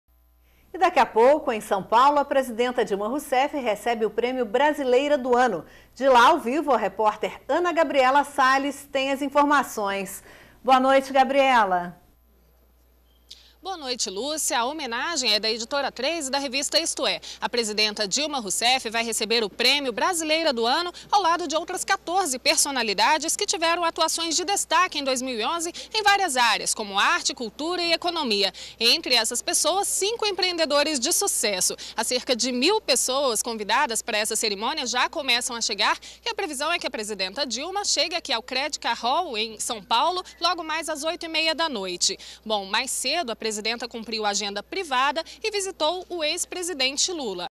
Dilma recebe Prêmio Brasileira do Ano, em São Paulo
NBR Notícias - 06.12.11: Em São Paulo, a presidenta Dilma Rousseff recebe o Prêmio Brasileira do Ano.